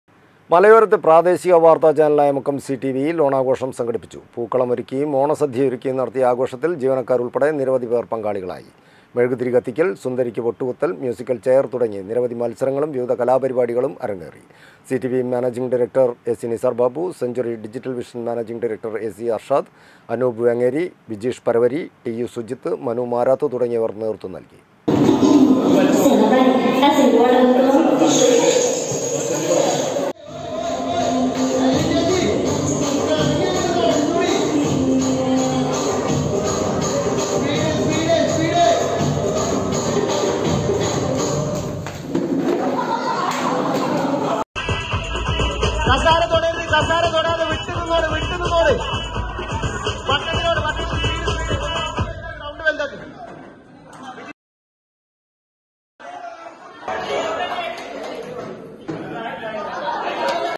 മലയോരത്തെ പ്രാദേശിക വാർത്ത ചാനലായ മുക്കം സി ടി വി യിൽ ഓണാഘോഷം സംഘടിപ്പിച്ചു
മലയോരത്തെ പ്രാദേശിക വാർത്ത ചാനലായ മുക്കം സി ടി വി യിൽ ഓണാഘോഷം സംഘടിപ്പിച്ചു. പൂക്കളമൊരുക്കിയും, ഓണസദ്യയൊരുക്കിയും നടത്തിയ ആഘോഷത്തിൽ ജീവനക്കാരുൾപ്പെടെ നിരവധി പേർ പങ്കാളികളായി.